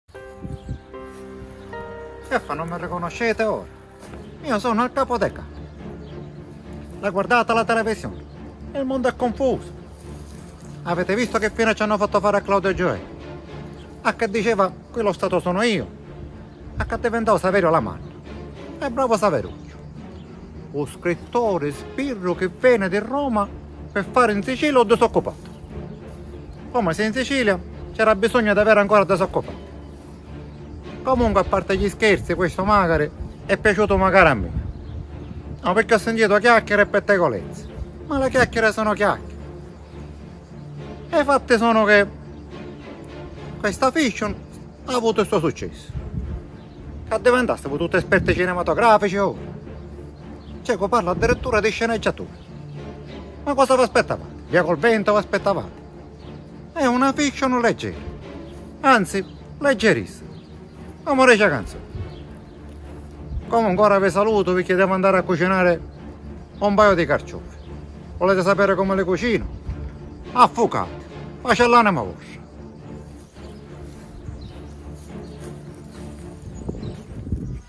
IL CAPO DEI CAPI, CLAUDIO GIOE' E MAKARI..PARODIA
IL CAPO DEI CAPI, (CLAUDIO GIOE'),  IMITATO DA ALESSANDRO GANDOLFO PARLA DI MAKARI, LA FICTION DI RAI UNO..